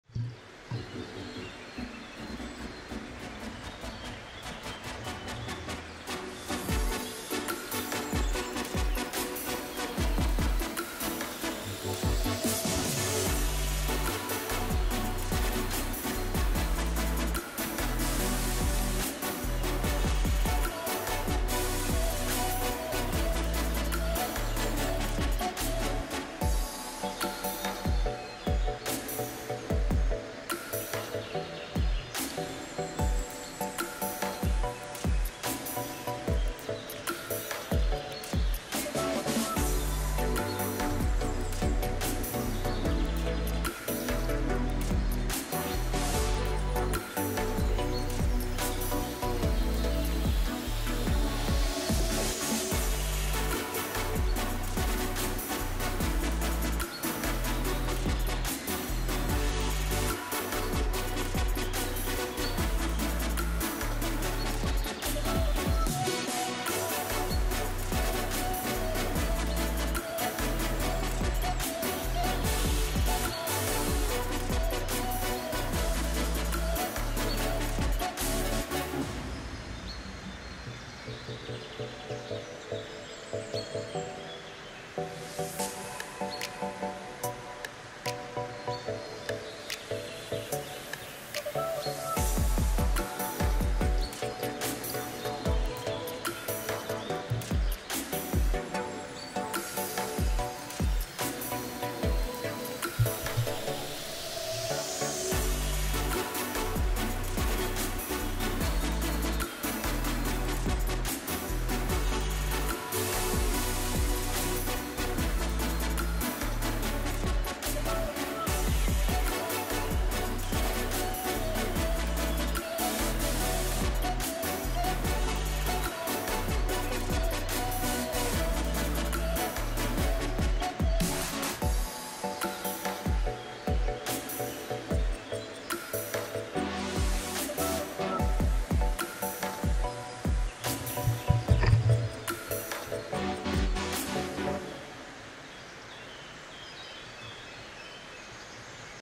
Bass Guarding Nest : Carp Chase Footage
In this video, I accidentally placed my underwater camera on a smallmouth bass nesting site while fishing for carp.
Watch as the protective bass fiercely chases away any carp that come too close to its nest.
It's a fascinating look at the natural behavior of these incredible fish.

The music used in this video is from Artlist.
If you're a video creator, be sure to sign up and enjoy making your videos!